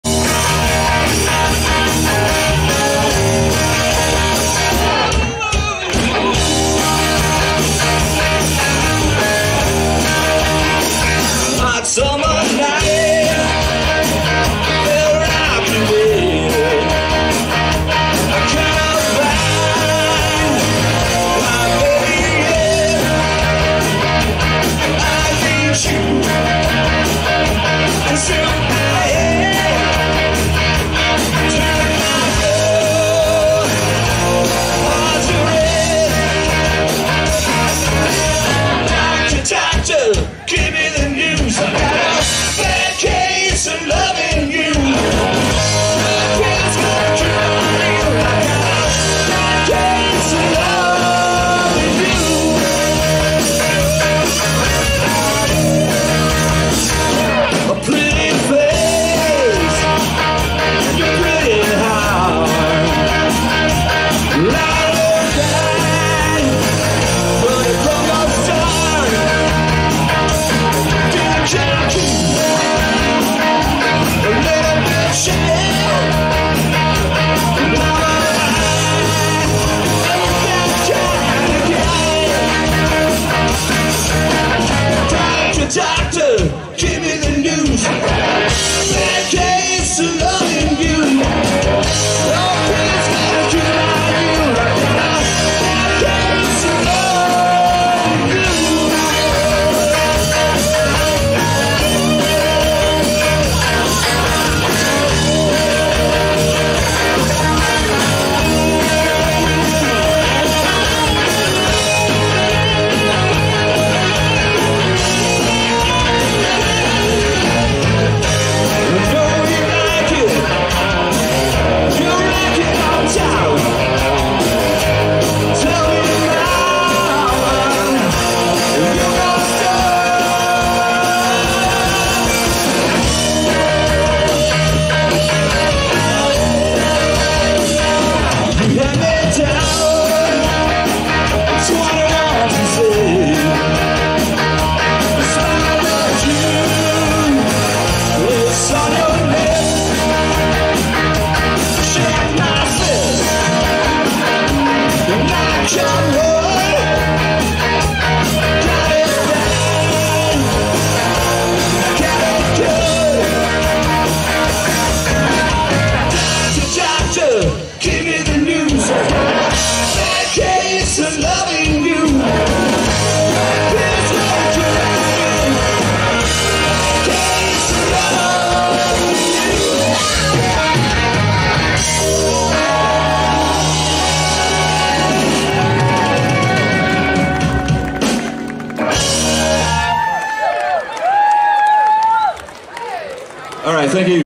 Angry Salad Band
Angry Salad Band Omaha - Video By Randys Photography Omaha
Taken At Cottonwood Cove Marina